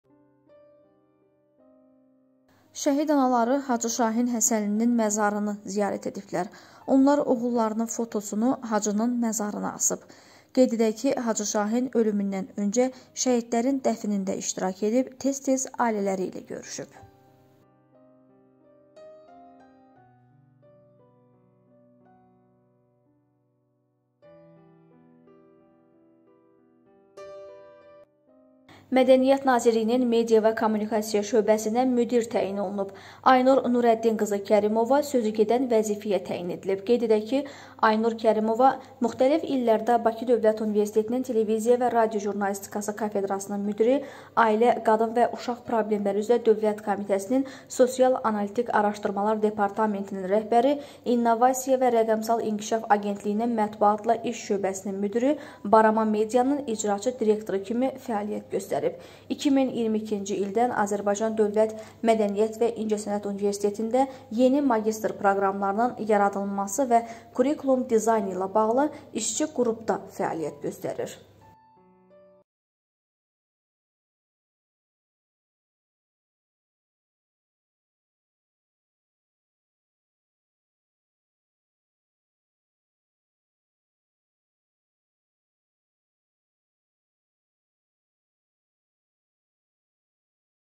Şəhid anaları Hacı Şahin Həsənlinin məzarını ziyarət ediblər və geriyə belə bir görüntü qalıb...
Hacı Şahin Həsənli dünyasını dəyişib
Hacı Şahin vəfat etdi - SƏBƏB
Reklam və Əməkdaşlıq üçün əlaqə nömrəsi: